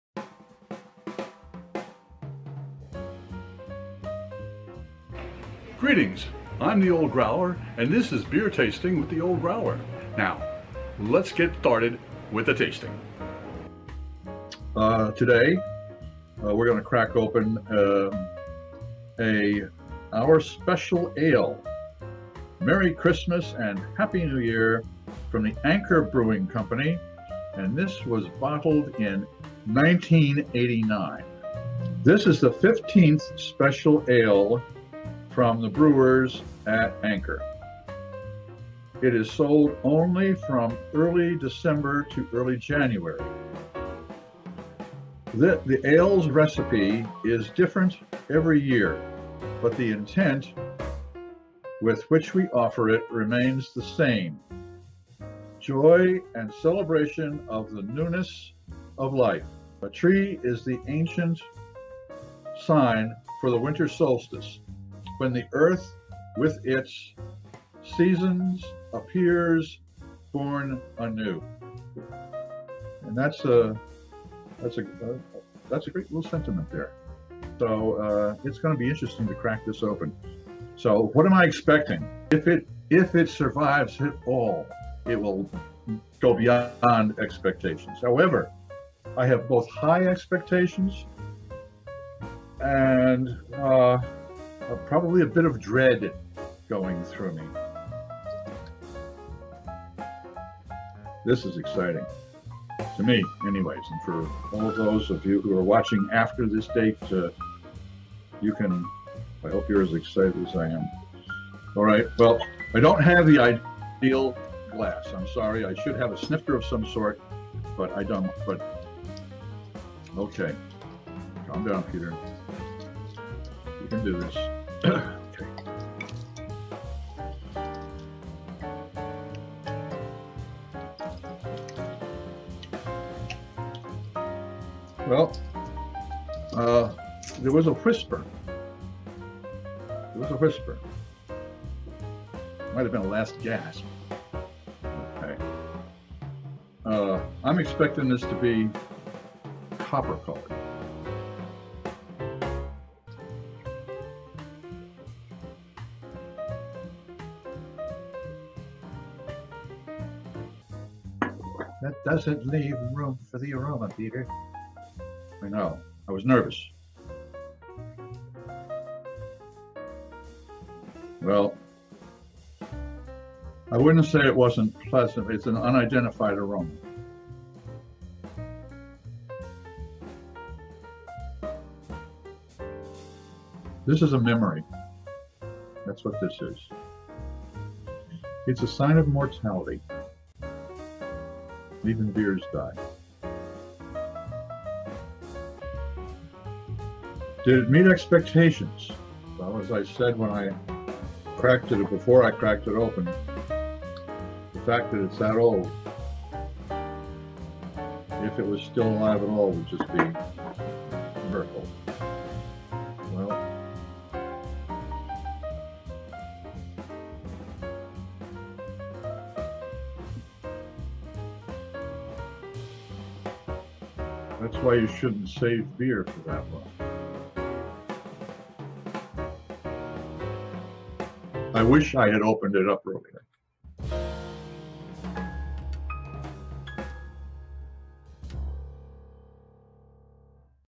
Anchor Our Special Ale 1989 - Beer Tasting with The Old Growler
Beer Tasting with The Old Growler   Anchor Our Special Ale 1989

This “Beer with The Old Growler” was recorded at 1700 hours (5:00 PM) on Thursday, 14 May 2020. It was the 59th day of New York State being under PAUSE.
This time the “Old Growler” samples a rare bottle of “Our Special Ale 1989” from the Anchor Brewing Co., San Francisco, CA.

The Anchor Brewery web site tells the whole story behind this beer.

“First brewed on November 17, 1975, Anchor Christmas Ale was not bottled until December 16, just nine days before Christmas. “As far as I could discover,” Fritz continues, it was “the only real Christmas brew in the world. The tradition had recently disappeared, I think, even in Scandinavia, where it had lingered.”

“One of the things I love most about Anchor Christmas Ale is the irony of its tradition. Rather than reprise the beer and the label year after year, Fritz chose to launch a tradition of newness and change. “The intent with which we offer it,” we say on our label, “remains the same: joy and celebration of the newness of life. Since ancient times, trees have symbolized the winter solstice when the earth, with its seasons, appears born anew.”

“At Anchor in 1991, Fritz received an unusual Christmas gift, a magnum of sparkling wine from fellow vintner Jack Davies, whose Schramsberg Vineyards was already well known for its effervescent creations. “I always admired Jack and visited Schramsberg early,” Fritz recalls. “He started in 1965 too, I think, and I sold him grapes for some of his first harvests. We had to pick in boxes for him (!), which was by then becoming very rare. His truck had a flat tire, which we fixed for him. Very amusing. And Napa Gamay grapes for his rose champagne.” Fritz would later make a York Creek Valdiguié (AKA Napa Gamay) in his winery across the street from the brewery.”

“Fritz could have just sent a 6-pack of Christmas Ale to Jack as a thank-you gift but that is not Fritz. He had a better idea. What if we were to bottle a little of our 1991 Christmas Ale in magnums so he could surprise Jack with a bottle and return the favor in kind? It wasn’t easy, but we did it.”

“I’ll always remember the tree on our 1991 label (a birch) because that was my first Christmas Ale as a member of the Anchor family. On December 12, we bottled 103 magnums (the goal was 100, but we had a couple extra bottles) of a keg in the cellar. At that time we did not yet have a bottling line that could handle this huge bottle, which is twice the capacity of a standard wine bottle and super heavy to accommodate the pressure of méthode champenoise sparkling wine.”

And from the “Old Growler,” a short Thank You.

Thanks to all of you that have watched this presentation. I would sure appreciate it.

Let me know what you think of this video.

Best wishes and Stay Safe!

Cheers,
Peter LaFrance